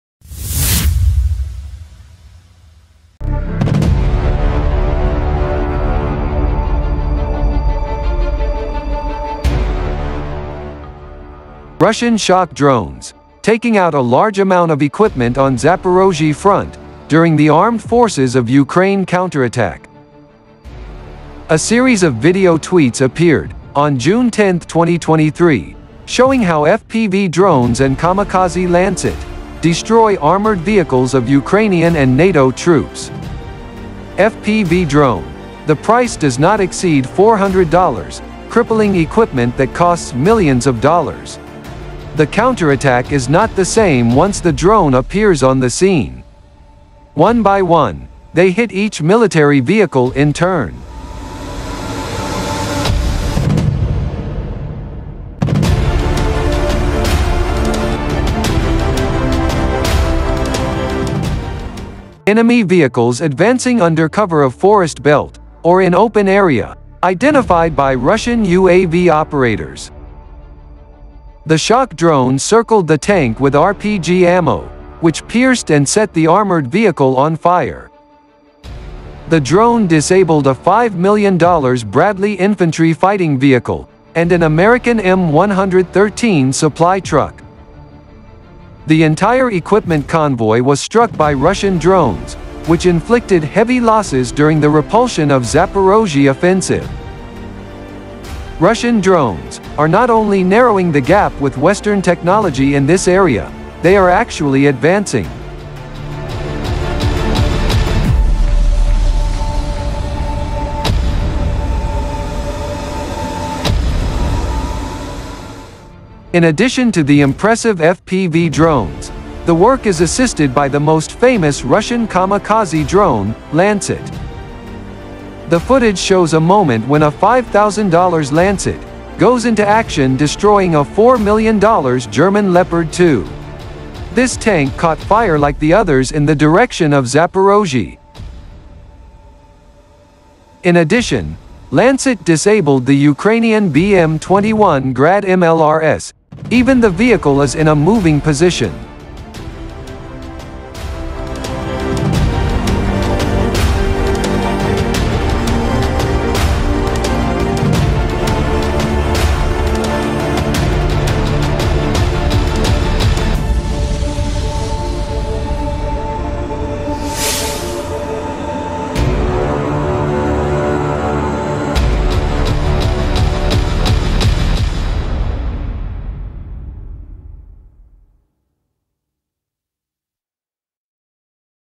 Russian shock drones taking out equipment on Zaporozhye front during the counterattack of Ukraine
Military Tube Today
10 Jun 2023

Russian shock drones taking out a large amount of equipment on Zaporozhye front during the counterattack of Armed Forces of Ukraine. Repelling attacks of Zaporozhye by FPV drones and kamikaze Lancet, one by one they emerged and hit every Ukrainian and NATO vehicle, costing millions of dollars such as American Bradley infantry fighting vehicle and German Leopard 2.